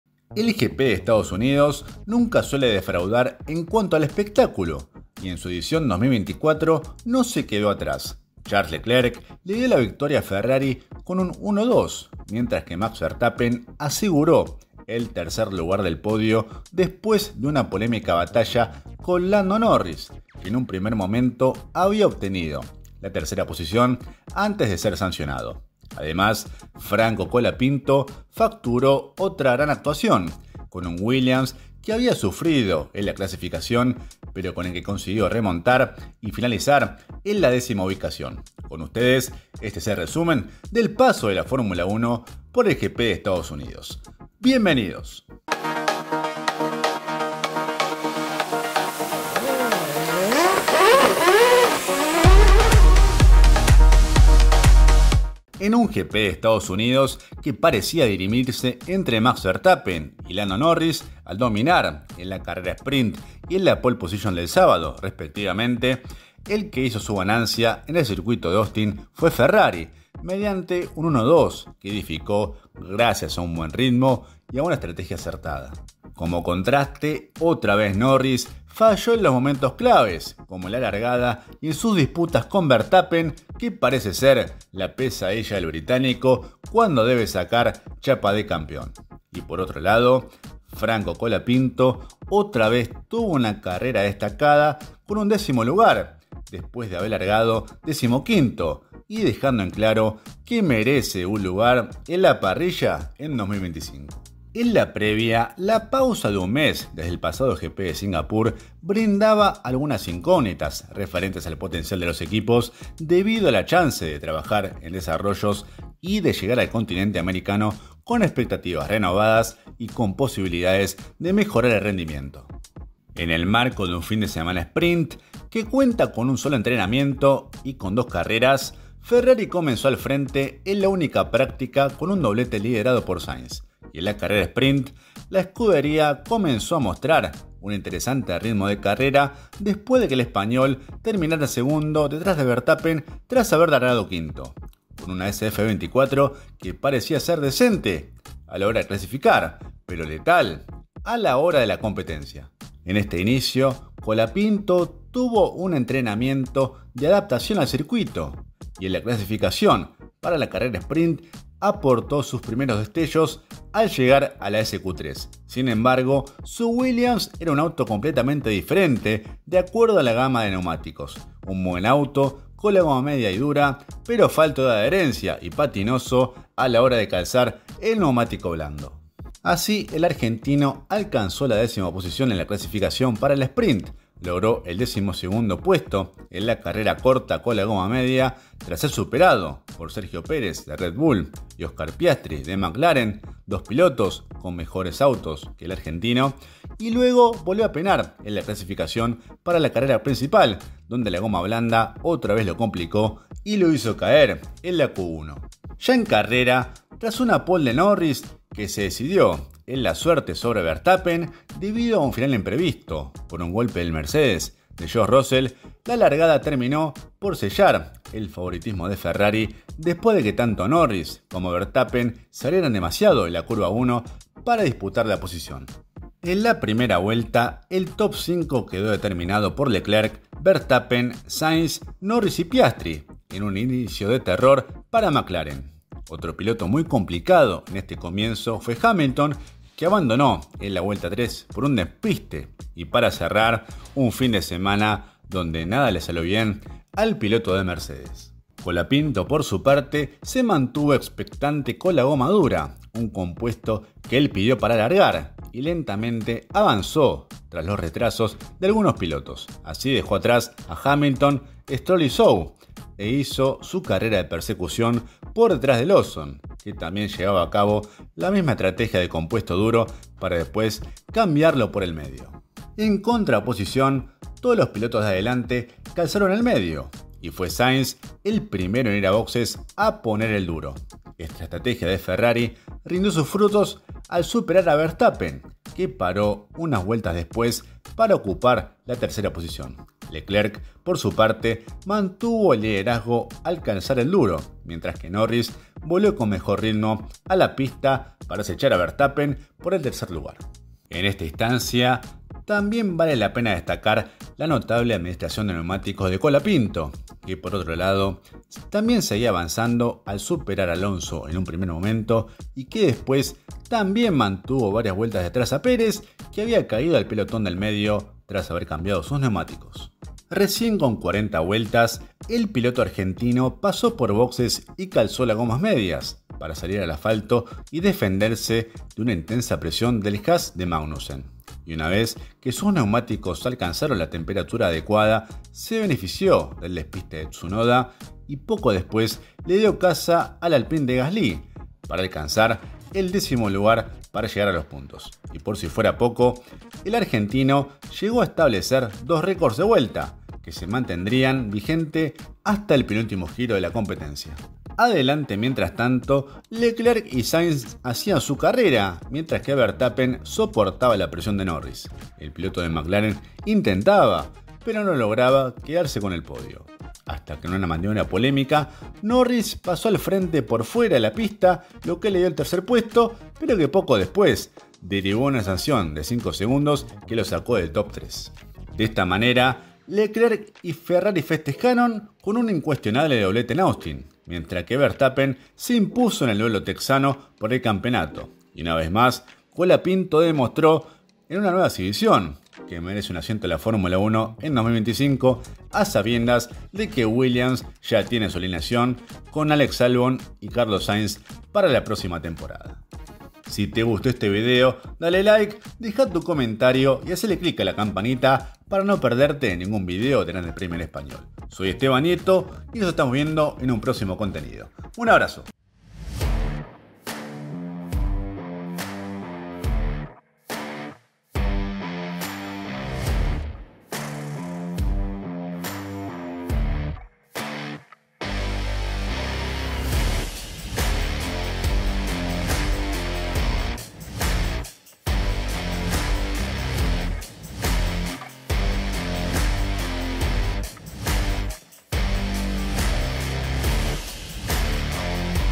RESUMEN - GP DE ESTADOS UNIDOS
El GP de Estados Unidos nunca suele defraudar en cuanto al espectáculo, y en su edición 2024 no se quedó atrás. Charles Leclerc le dio la victoria a Ferrari con un 1-2, mientras que Max Verstappen se aseguró el tercer lugar del podio después de una polémica batalla con Lando Norris, que en un primer momento había obtenido la tercera posición antes de ser sancionado.

Además, Franco Colapinto facturó otra gran actuación, con un Williams que había sufrido en la clasificación, pero con el que consiguió remontar y finalizar en la décima ubicación.

Con ustedes, este es el resumen del paso de la Fórmula 1 por el GP de Estados Unidos.